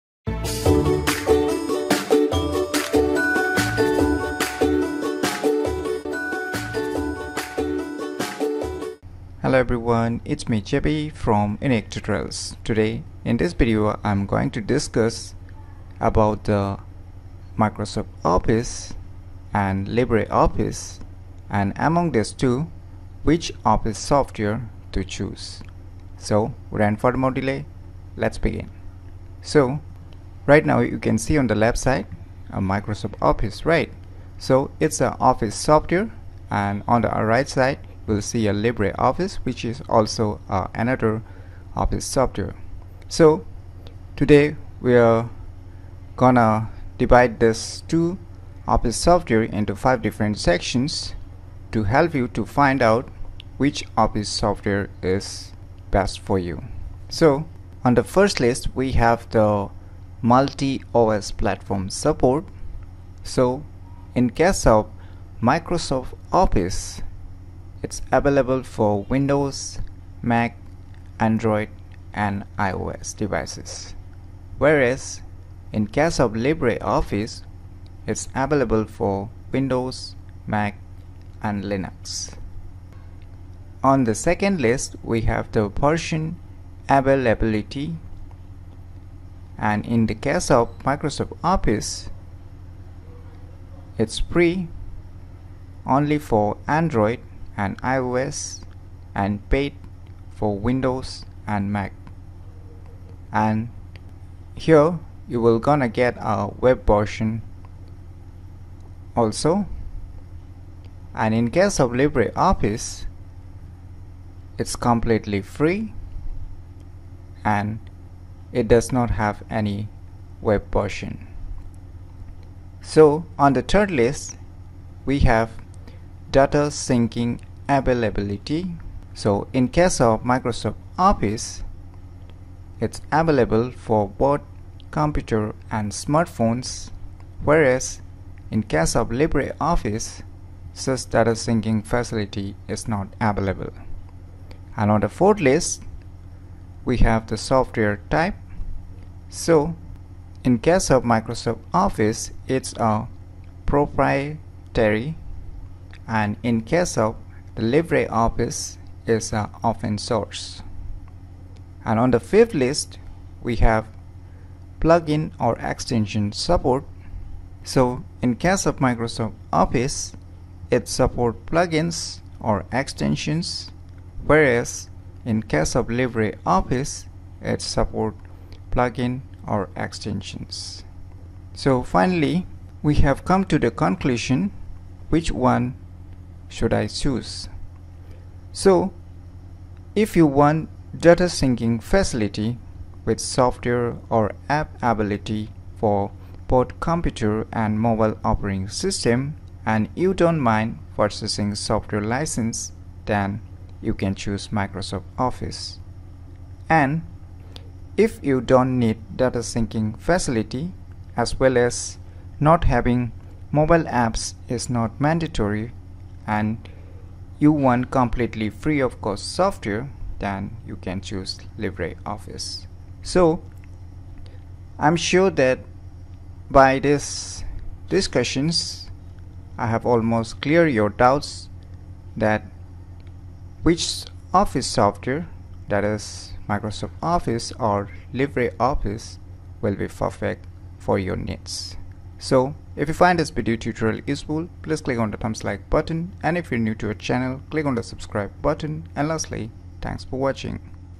Microsoft Office or Libre Office - Which Office Software to choose ?
This video tutorial is all about Microsoft Office or Libre Office - Which Office Software to choose.